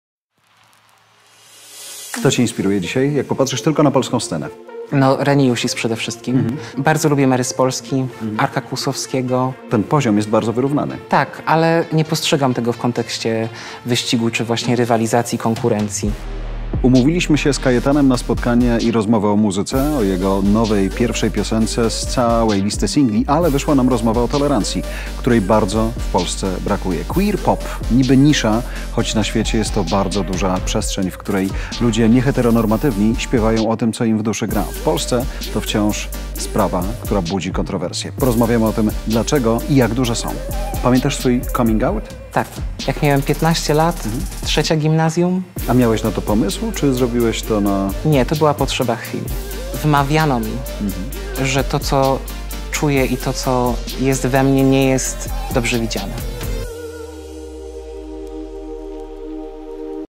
Perlage Stories by Jarosław Kuźniar | Kajetan Kopala
To miała być rozmowa wyłącznie o muzyce, ale okazało się, że to za mało. Thekayetan to przecież muzyk zaangażowany, który celuje w niszę - queer pop. Jego teksty często nawiązują do trudności z jakimi borykają się środowiska LGBTQ+ w Polsce.

Gościem Jarosława Kuźniara w cyklu PerlageStories jest Kajetan Kopala - kompozytor, producent muzyki elektronicznej i autor tekstów. To będzie rozmowa o równości, o muzyce, która staje się ważnym głosem dla społeczności osób nieheteronormatywnych i o tym, jak wiele może zmienić nowe pokolenie.